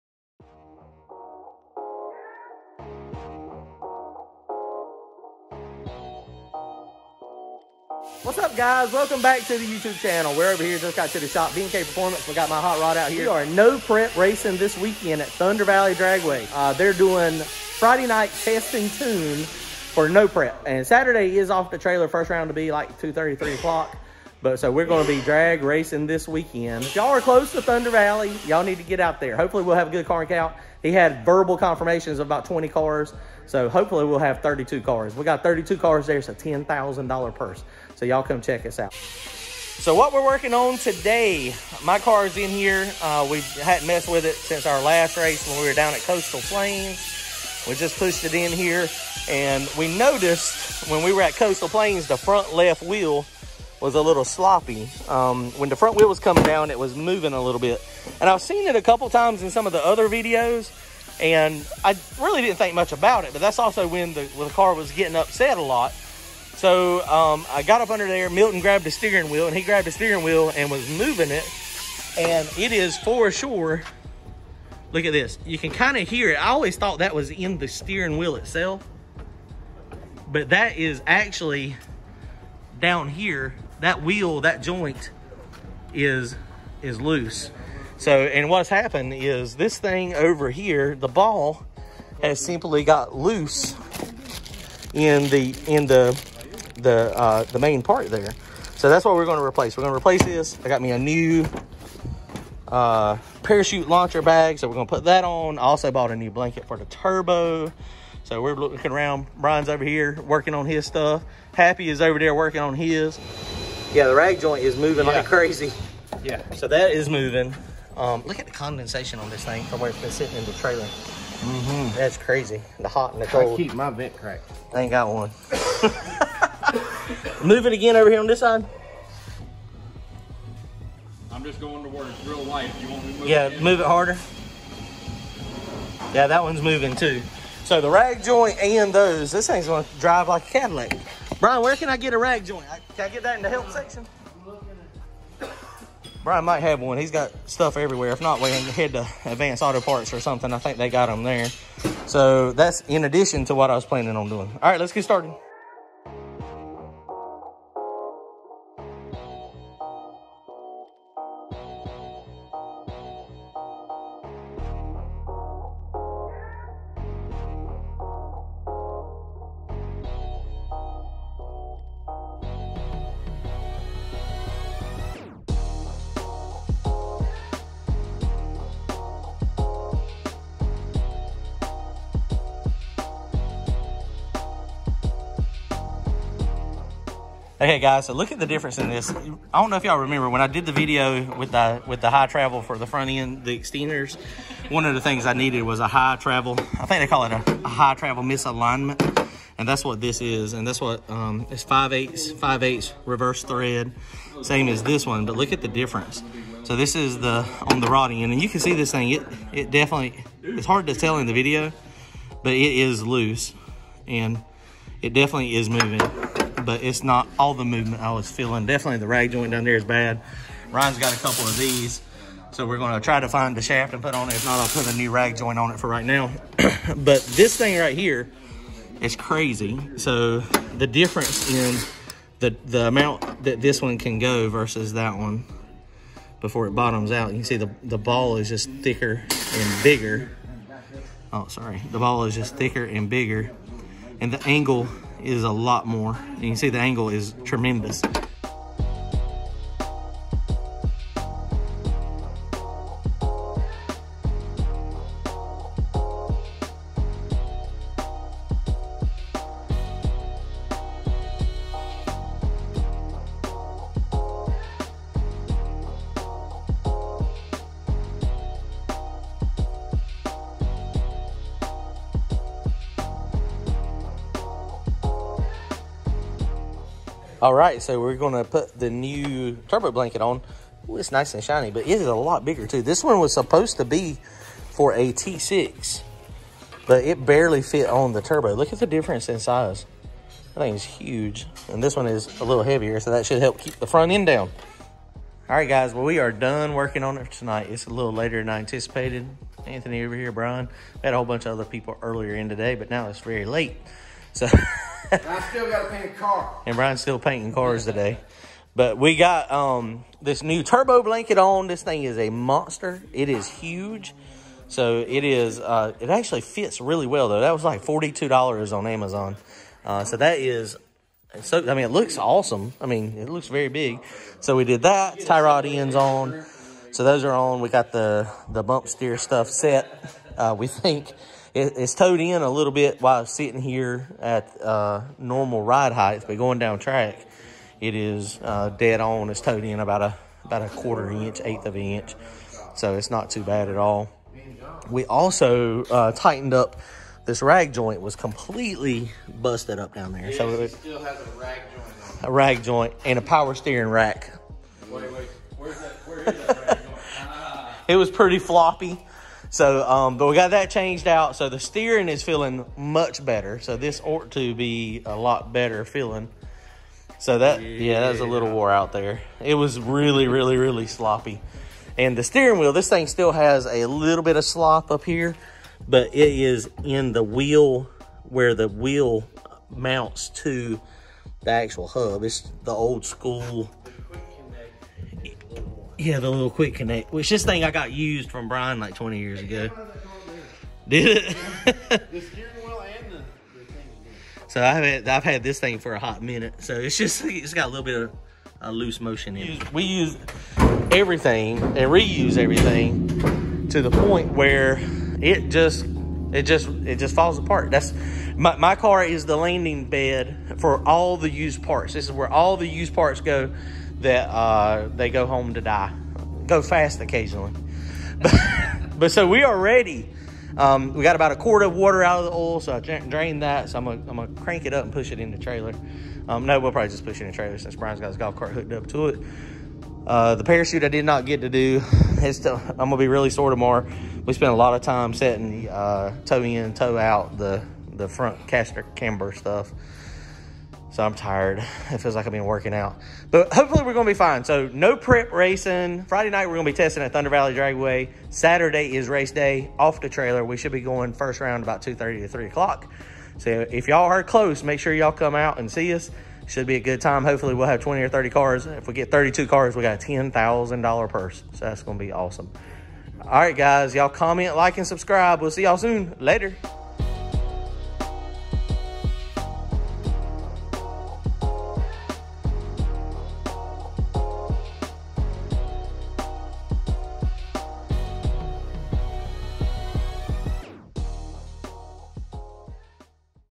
Getting car prepped to go racing!
Doing some maintenance on this thing trying to get it ready to go no prep racing this weekend.  Gonna be a great time.  ThunderValley Dragway, 10,000 dollar purse, come see us!

Hang out with me live every Wednesday night for Tech Talk! Ask questions and share your setups.